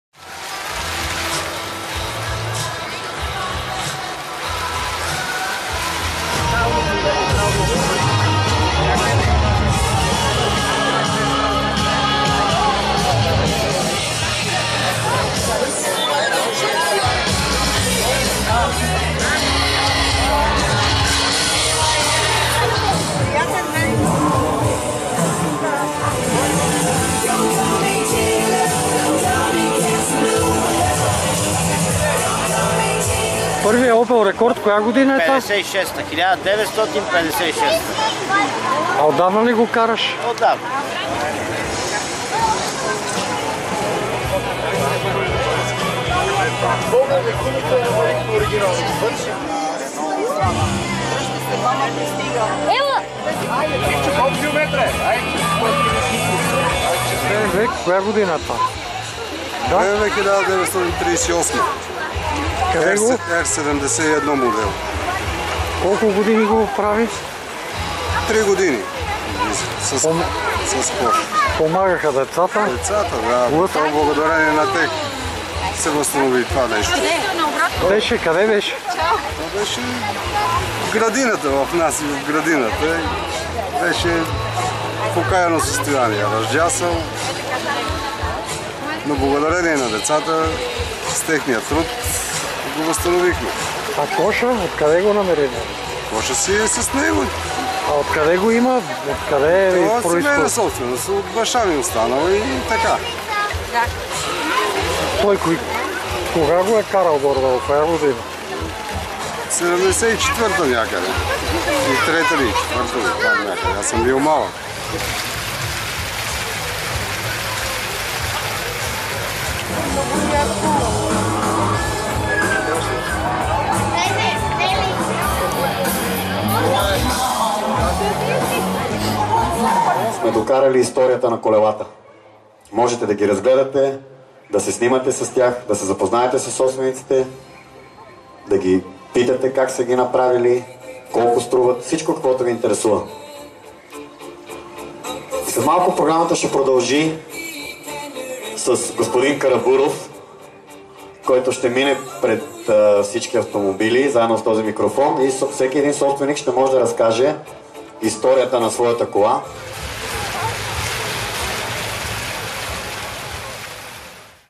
Мерцедеси и Варшави на равна нога на първия ретро-парад в Дупница
„Мерцедес-Бенц”-кабрио с автоматични скорости, производство 1958 г. и мотоциклет БМВ, „роден” през далечната 1938 г., „събраха очите” на познавачи и зяпачи на първия ретро-парад, провеждащ се днес в Дупница. /БГНЕС